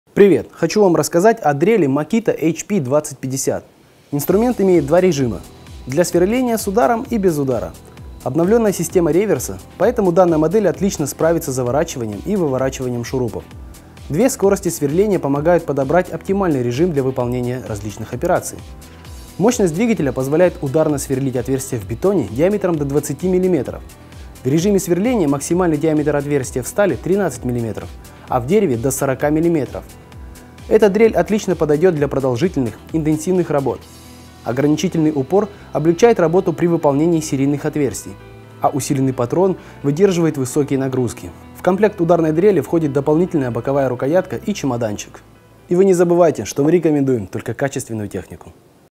Дрель ударная MAKITA HP 2050
Дрель ударная MAKITA HP2050 720Вт 13мм 0-1200/0-2900об/мин кейс

- Расцепляющая муфта защищает пользователя и инструмент при заклинивании сверла
- Электронная регулировка числа оборотов
- Высокая скорость сверления при малых вибрациях
- Прочный компактный корпус редуктора из литого под давлением алюминия
- Выключатель с большой кнопкой и удобным переключением реверса
- Механическое переключение 2 скоростей вращения